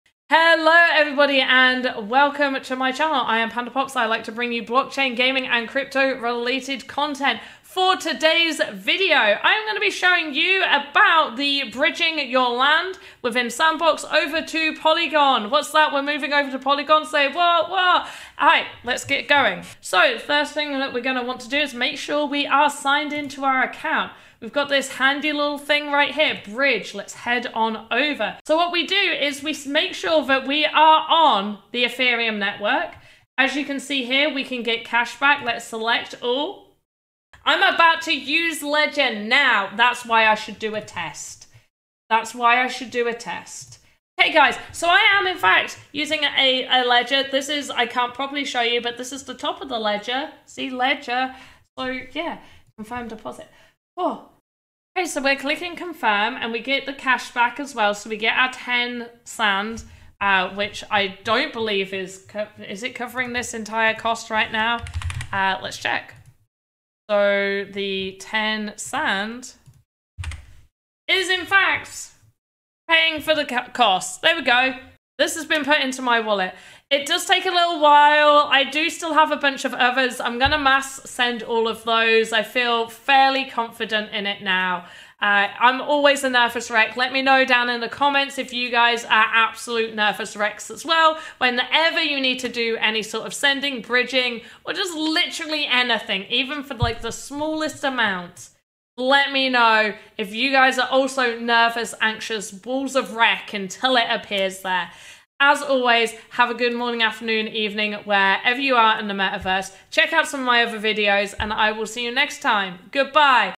Guide How To Bridge Sandbox Land To Polygon
Join me as I go through the steps to Bridge your Sandbox Game Land over from ETH to Polygon.

The bridging process also gives you 10 SAND on the Polygon Network.